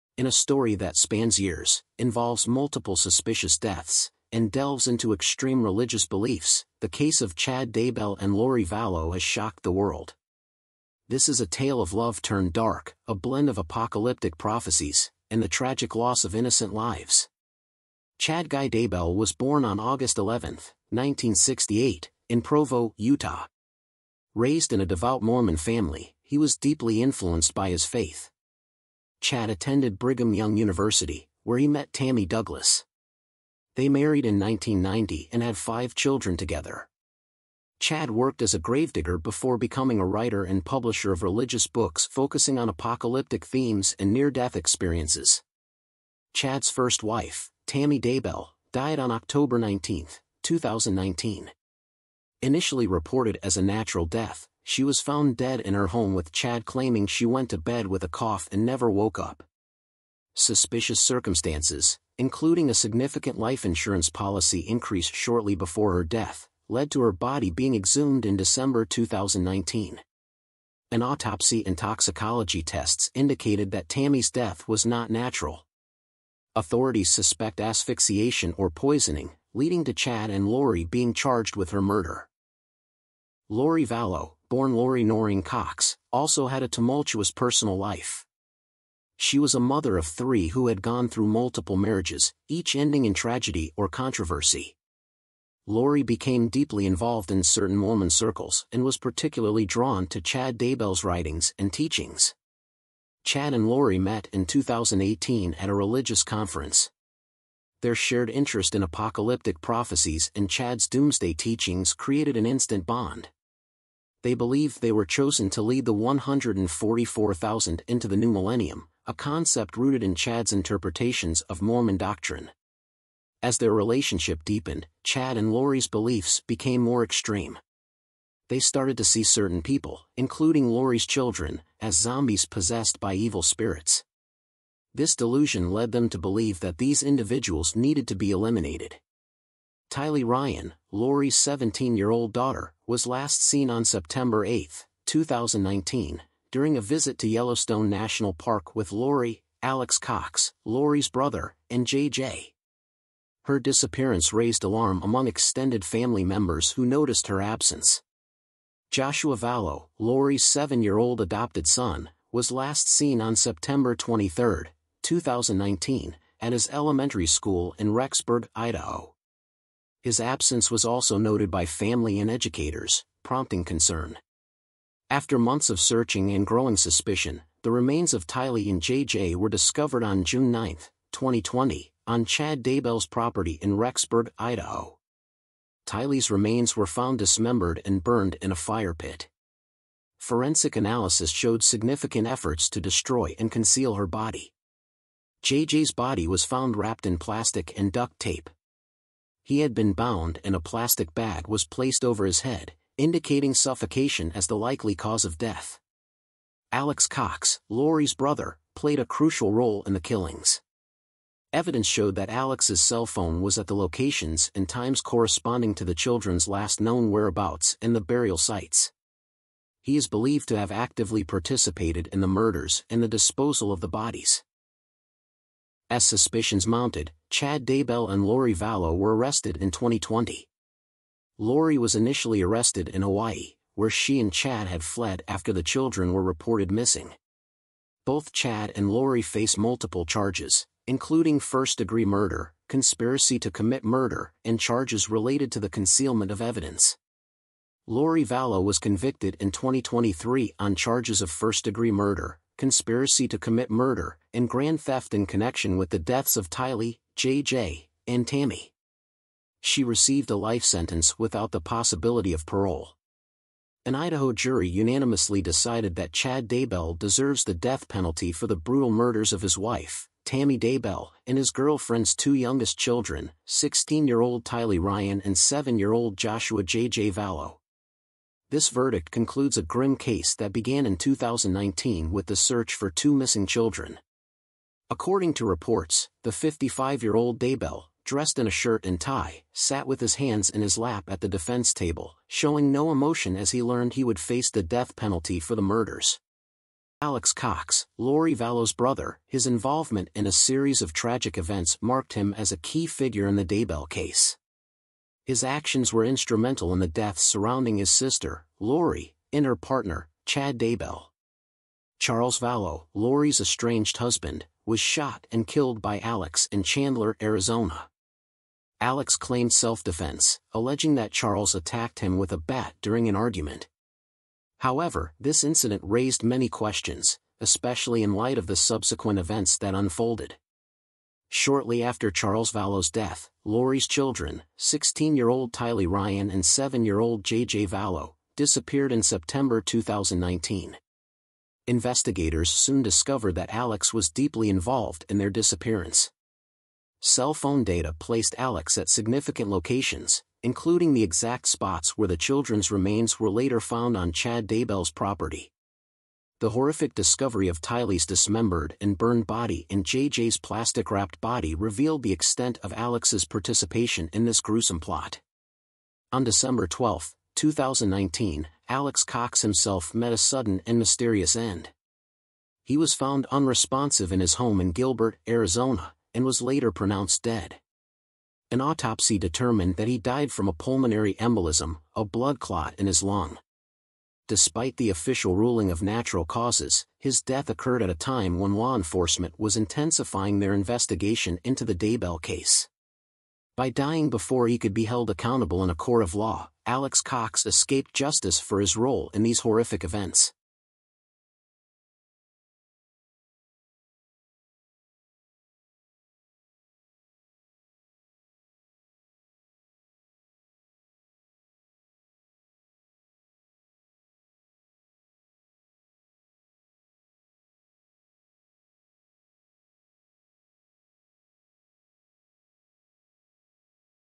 Zombies, Lies, and Murder- Daybell Case| True Crime
Dive into the chilling true story of Lori and Chad Daybell, a couple whose extreme beliefs in doomsday prophecies and "zombies" led to the tragic murders of Lori's children, Tylee Ryan and JJ Vallow, and Chad's wife, Tammy Daybell. This documentary unravels their descent into madness, the horrifying discoveries, and the gripping investigation that brought their sinister actions to light. Watch as we explore the twisted world of the Daybells and the devastating impact on those around them.